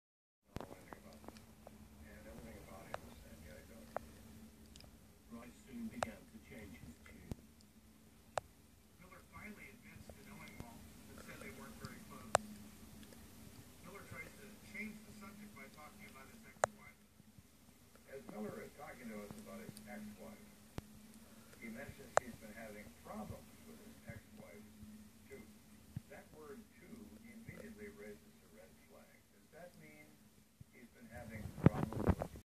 Electric blue ram eating zucchini/courgette
Monty loves his zucchini